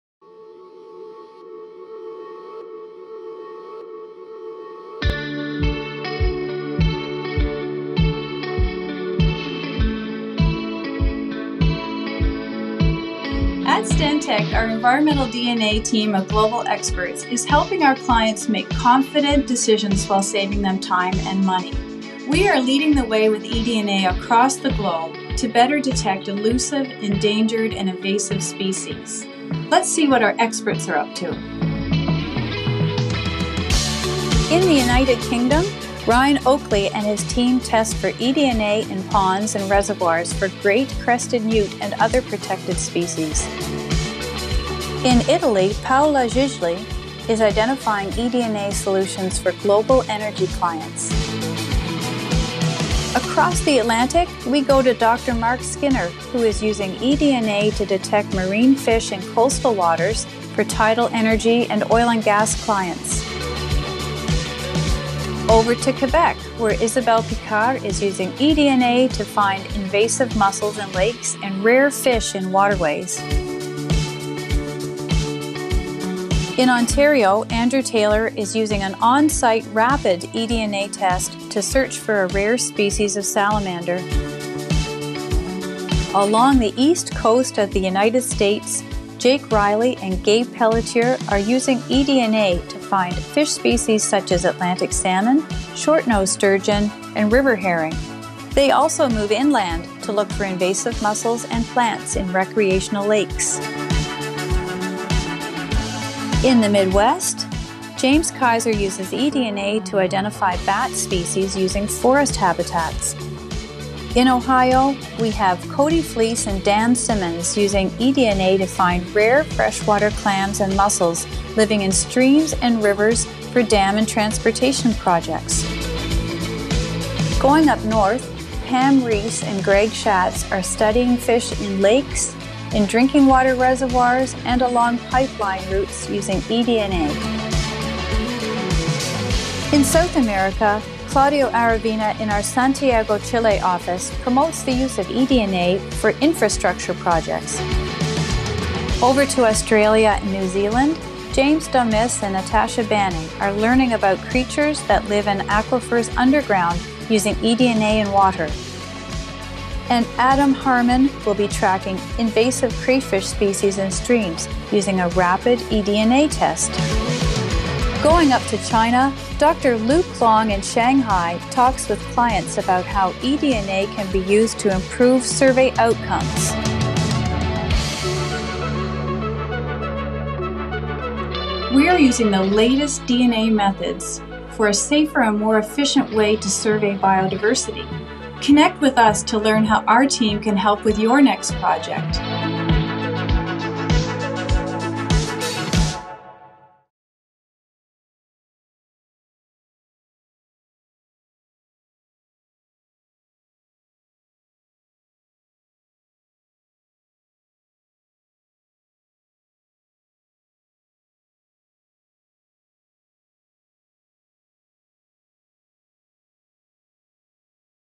Líderes globales en aplicación del eDNA
Nuestro equipo global utiliza el ADN ambiental (eDNA) para detectar especies críticas sin la necesidad de verlas o capturarlas. Nuestro uso novedoso del eDNA es más seguro, rápido y menos costoso, y ayuda a preservar la biodiversidad.